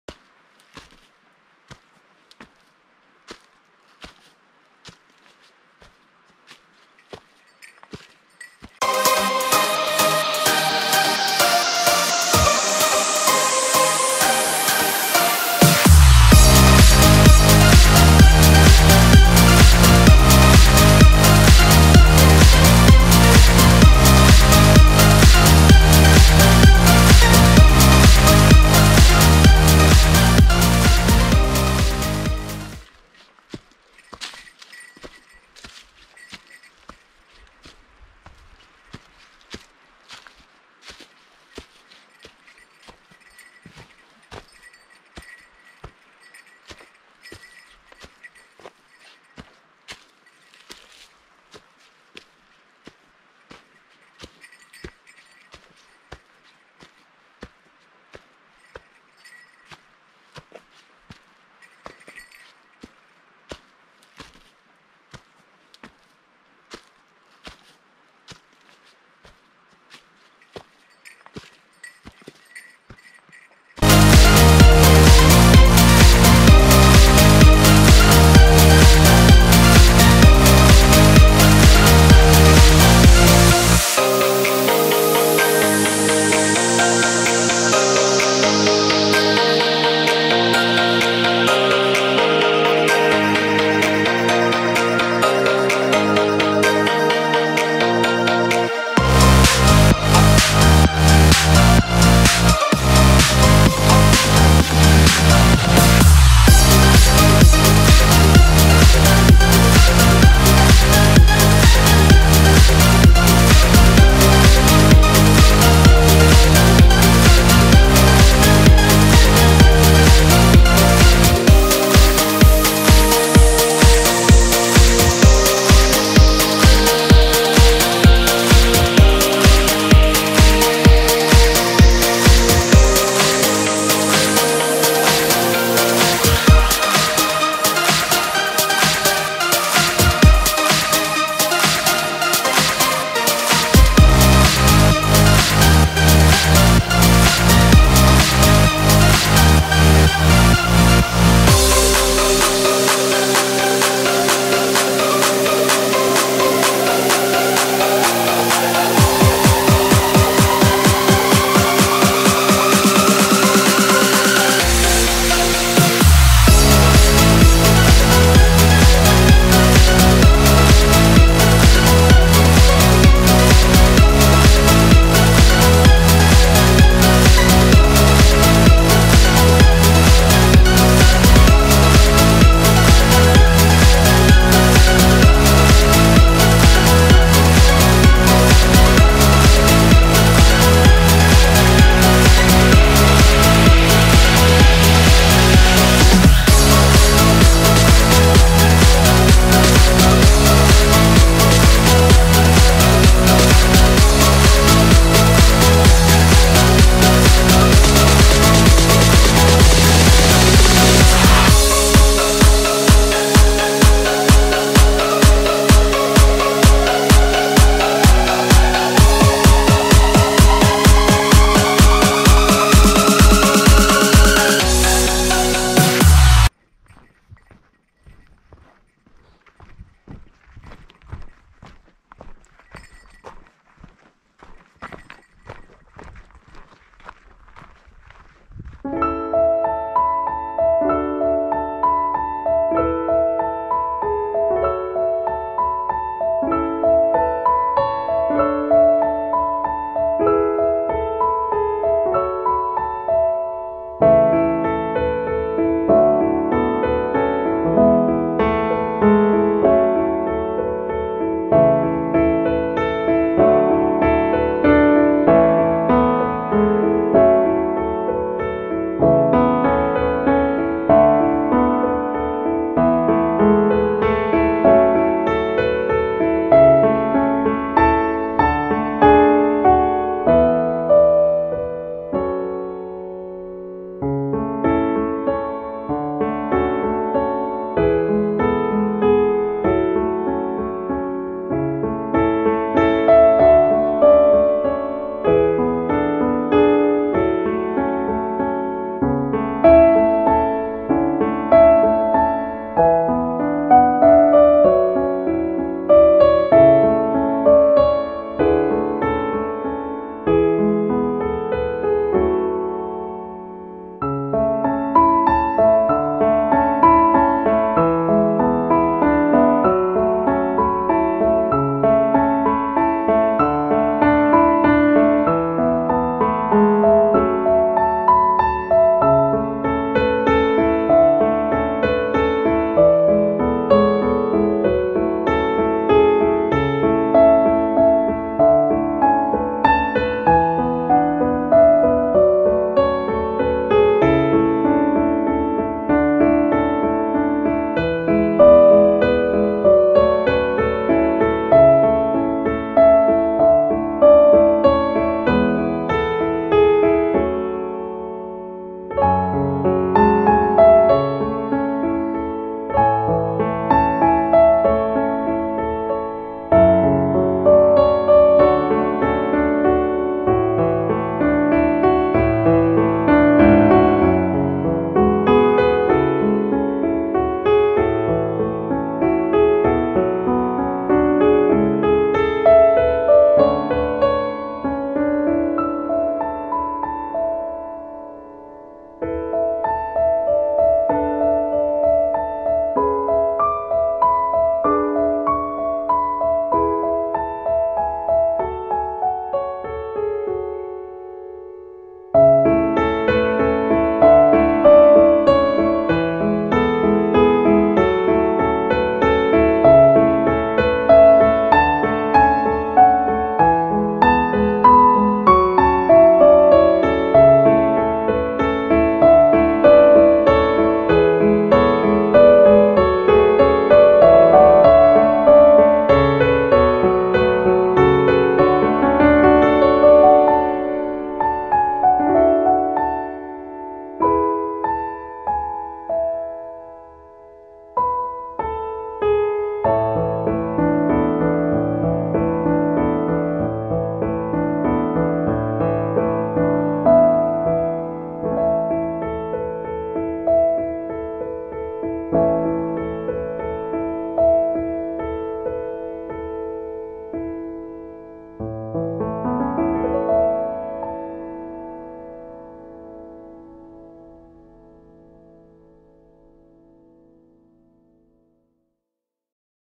錦秋の金剛堂山(栃谷~奥金剛↑↓)
紅葉の季節。２００名山の金剛堂山を栃谷登山口~前金剛~中金剛~奥金剛。山頂からは北アルプスや白山の眺望
撮影は主にGoPro hero 8 black。